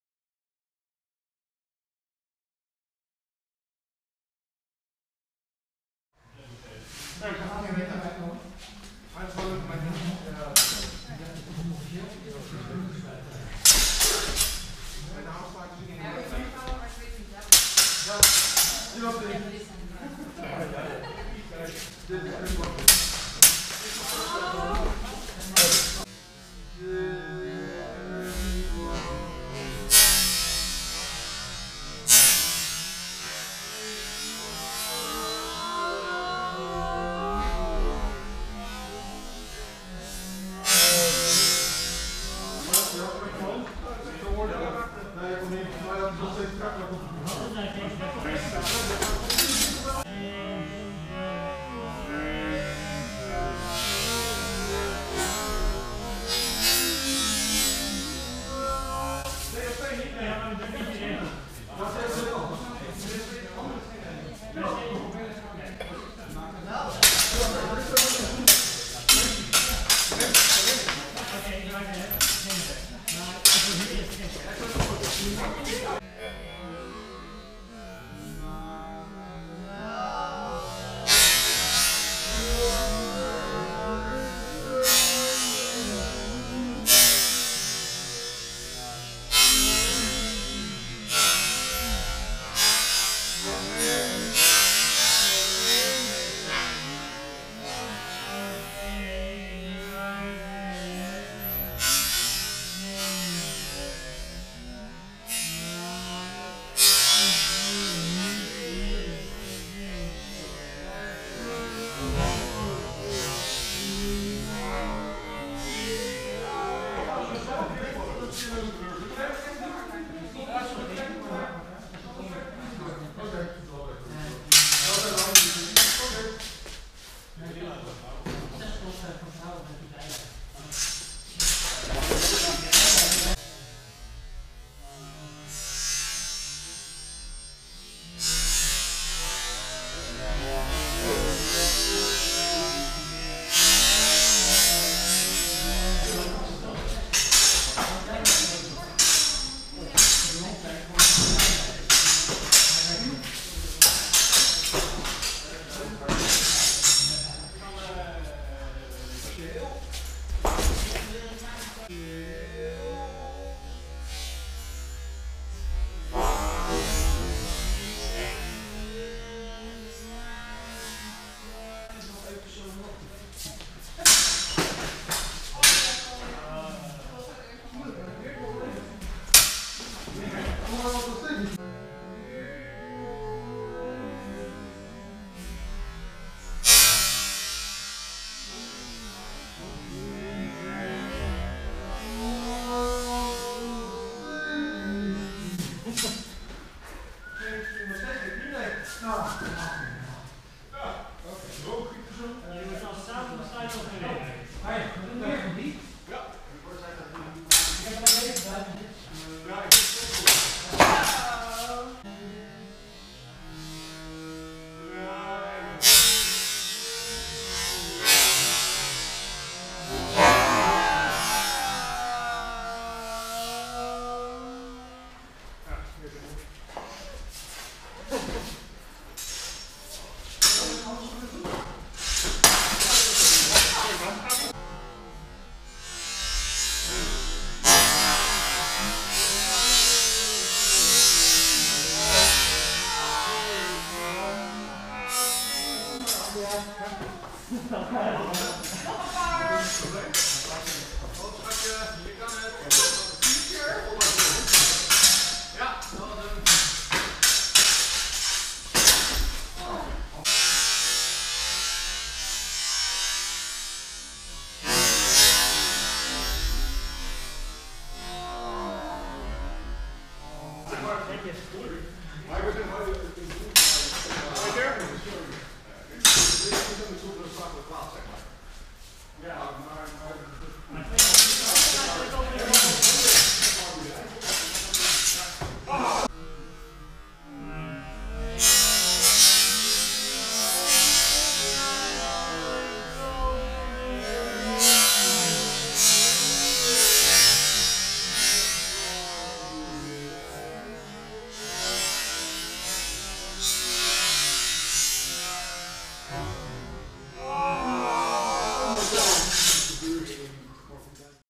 Summer Tournament - Arjen H vs Jos - Longsword
The Zwaard & Steen summer tournament of 2017. Our summer tournaments are very low-key. This year, the rules were as follows: people could challenge anyone and the match would go on until both fighters agreed on a winner (for instance, after a very clear hit). The winners received 2 points, the loser 1 point. Whoever accumulated the most points at the end of the day could pick out a prize from a table full of prizes.